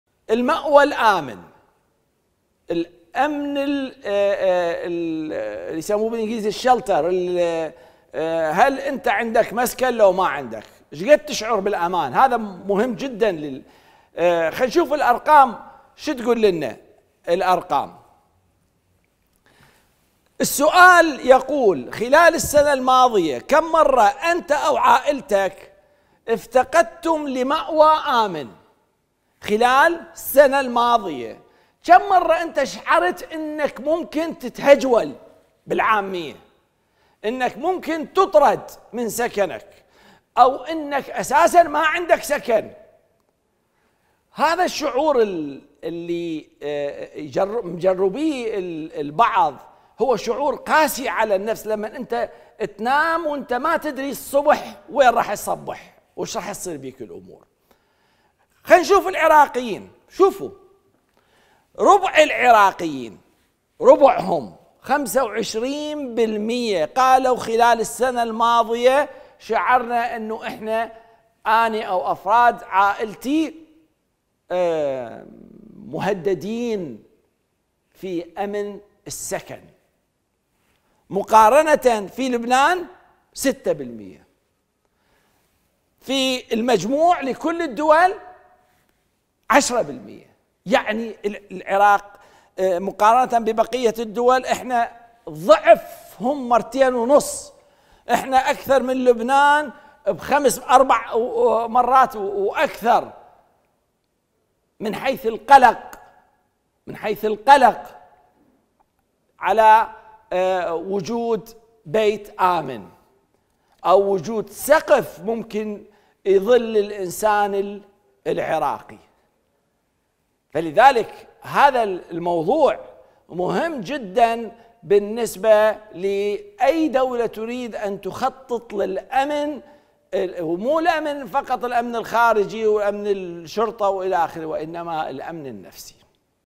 تعرف على نسبة العراقيين الذين افتقدوا المأوى الأمن خلال العام الماضي - رأيكم مع منقذ
بماذا يختلف العراقيون عن دول المشرق العربي؟